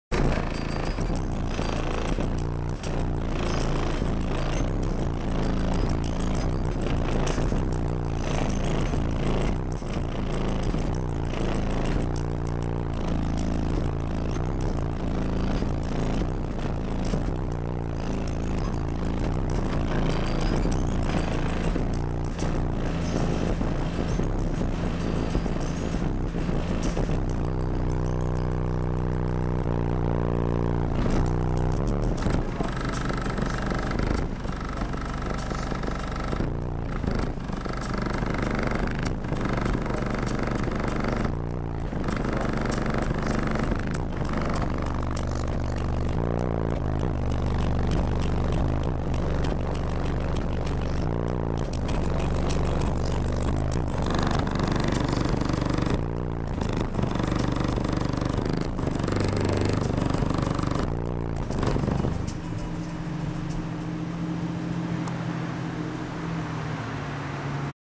Shirt ripples to Bass
Threw my shirt in the door to see how it would react to abit of afternoon bass and this was the result!!

Setup: 4 DD 515's on a DD M4 monoblock